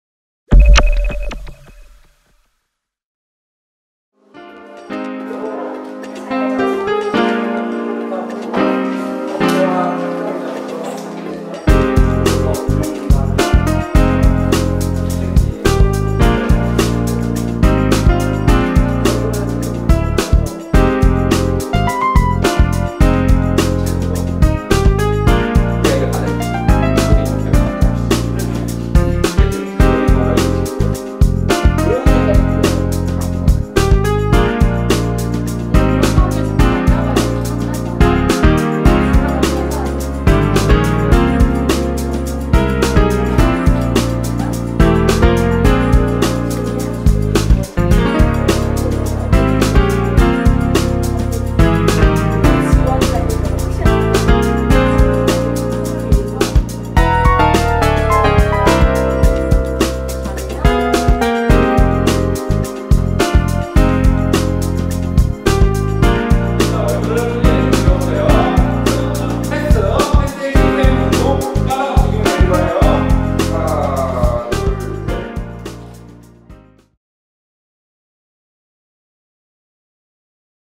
[싹튜브] 서울종합예술실용학교 방송영화 서고방연 미디어 캠프 (서울시 고등학교 방송반 미디어 진로 체험)
서울종합예술실용학교 방송영화 서고방연 미디어 캠프
(서울시 고등학교 방송반 미디어 진로 체험)